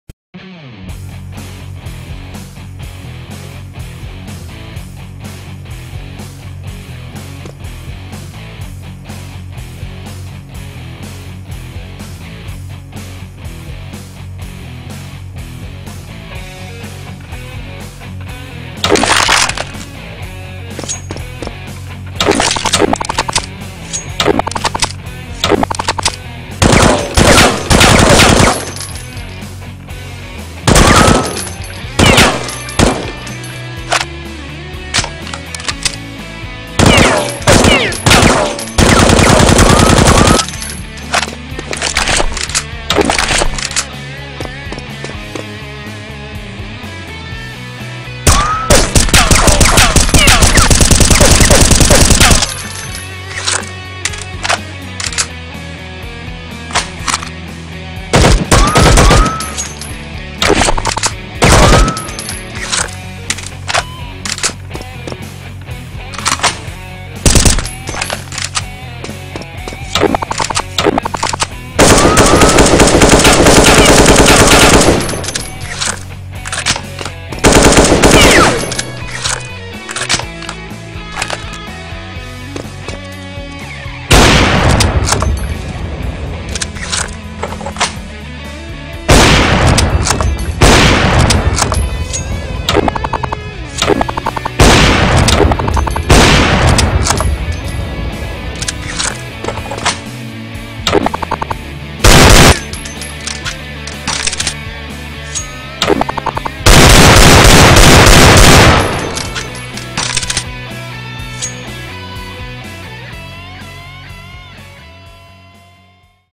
SHOWCASE LIONFISH Minipack
LIONFISH Minipack + default gloves
Include
m4a1-s
ak47
famas
deagle
AWP
Knife

if you like this work and want to purchase it, use these my contacts
SKYPE - olaf757575